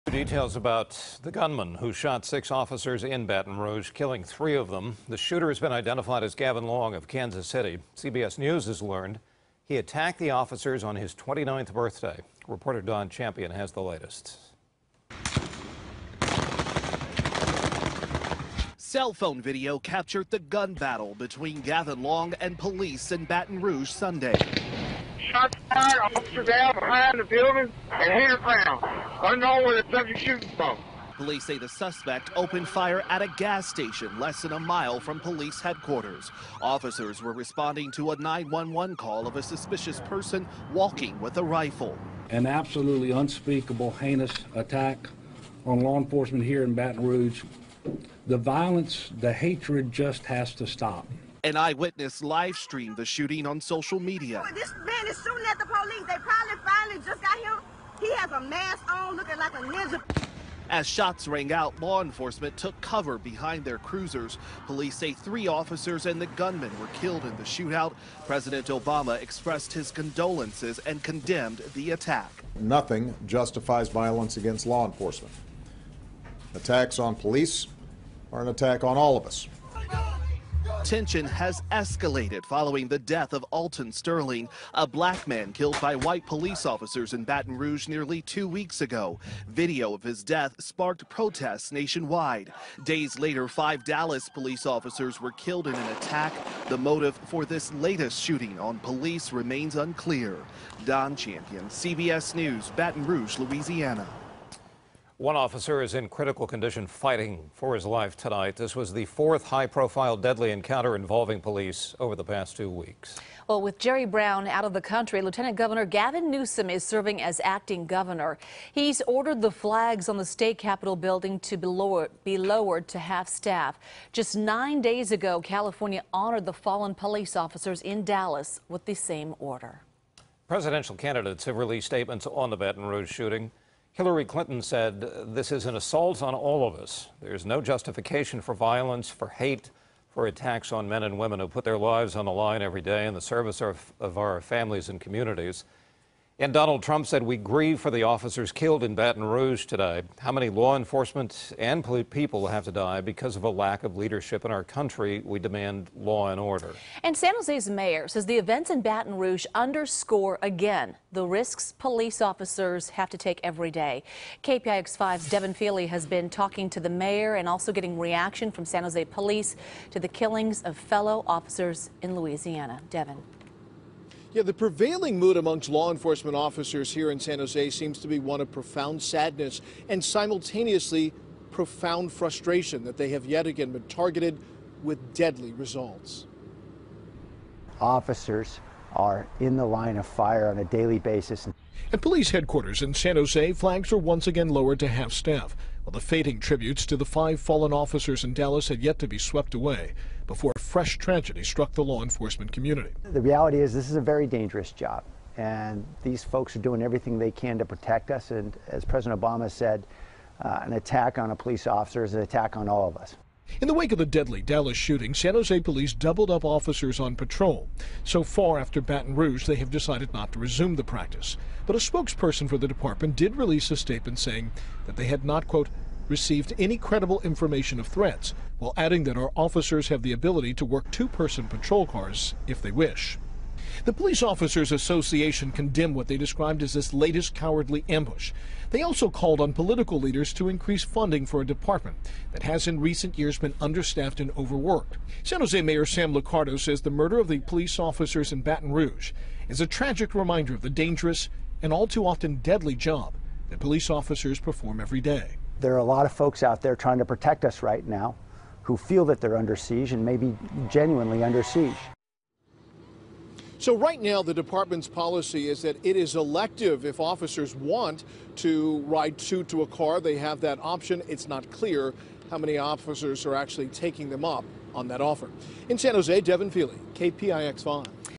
Team Coverage: Baton Rouge Officers Targeted In Shooting
A targeted shooting left three law enforcement officers dead in Louisiana Sunday.  Don Champion and Devin Fehely report.  (7/17/16)